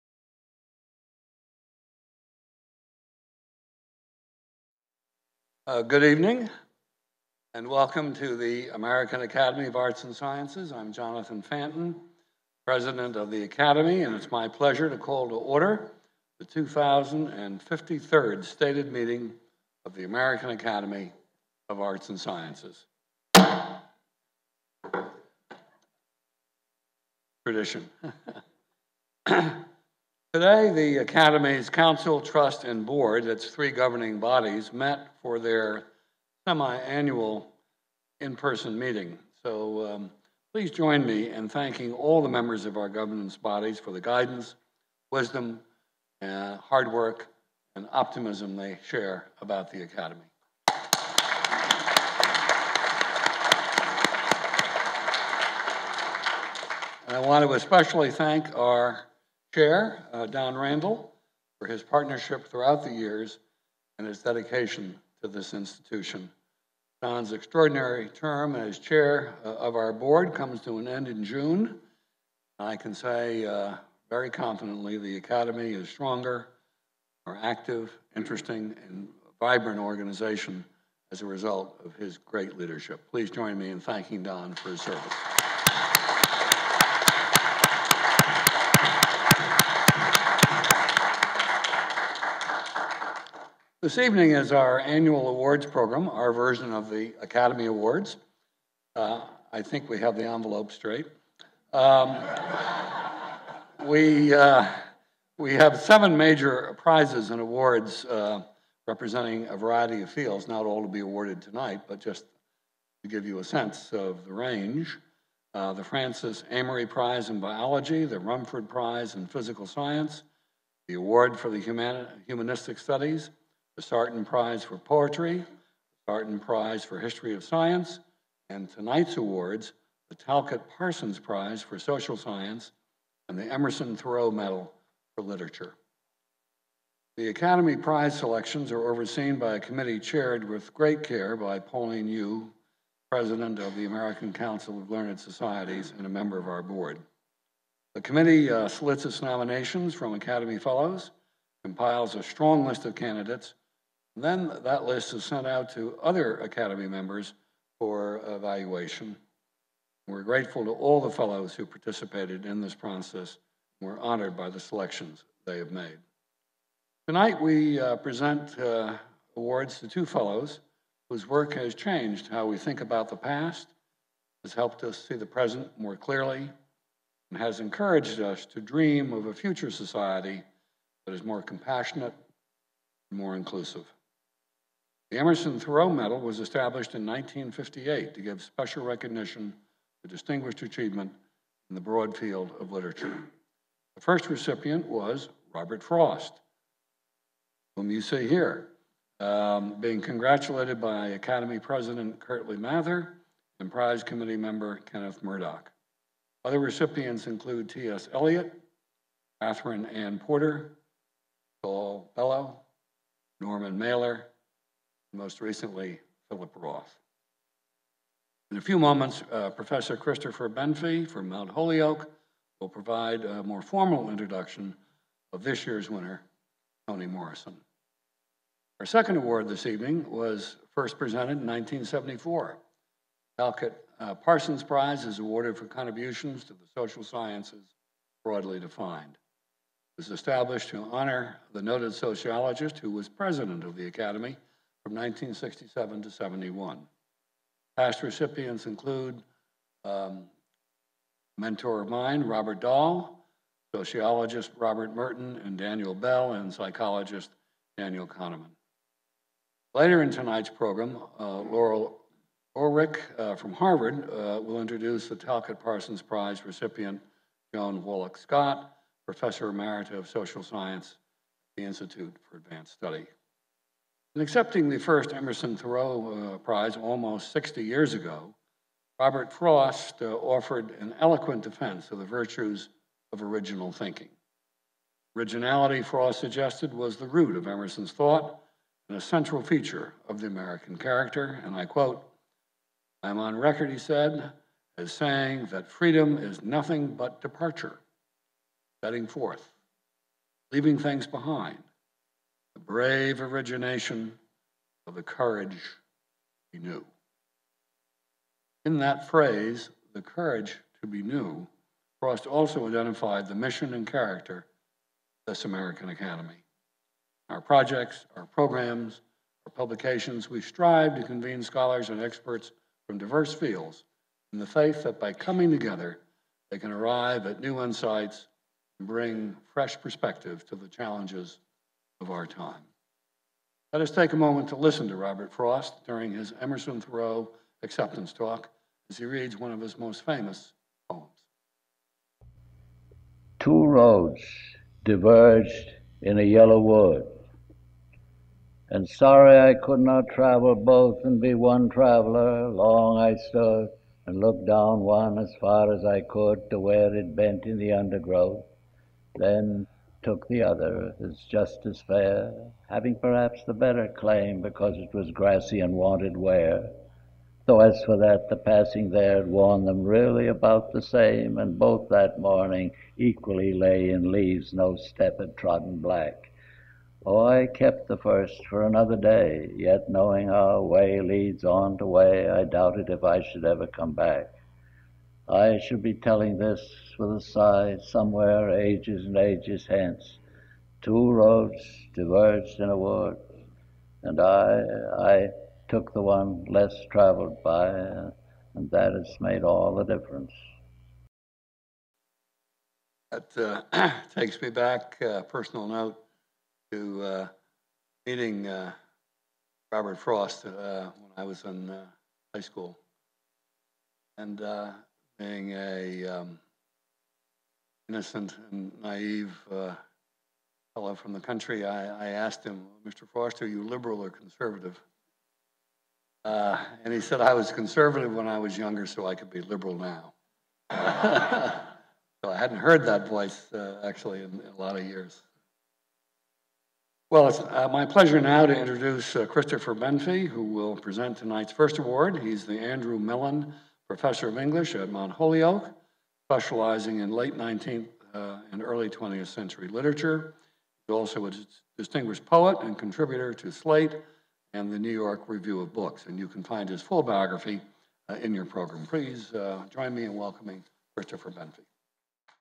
A Celebration of the Arts and Sciences - Jonathan F. Fanton
Jonathan F. Fanton, President of the American Academy of Arts and Sciences, welcomes everyone and calls to order the 2053rd Stated Meeting of the Academy.